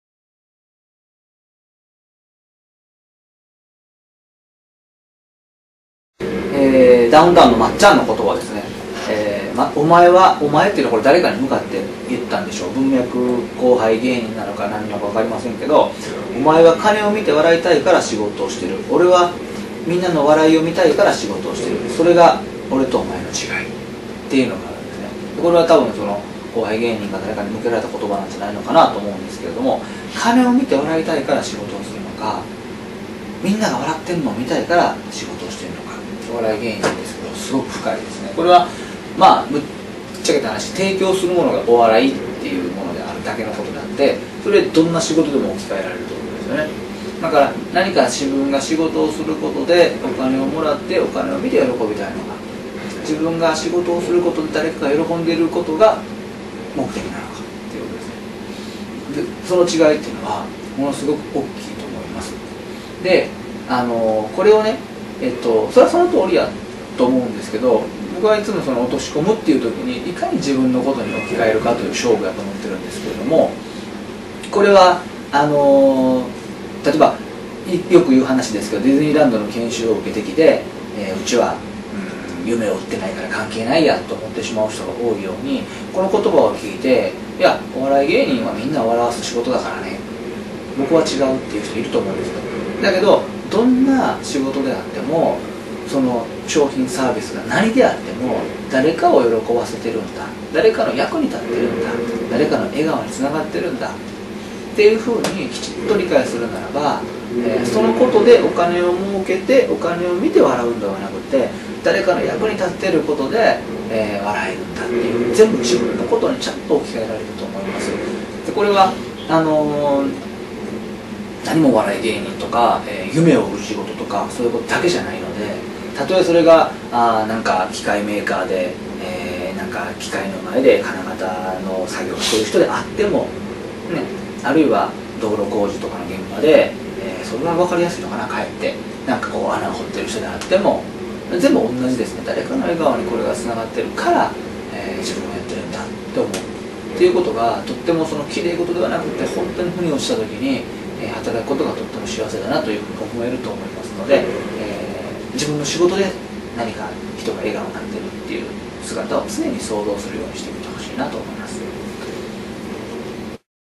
名言・格言の活かし方～ダウンタウン松本～
竹谷泰史さんが、名言・格言を自分自身への活かし方を、お話になります。今回は、ダウンタウン松本さんの有名なお言­葉です。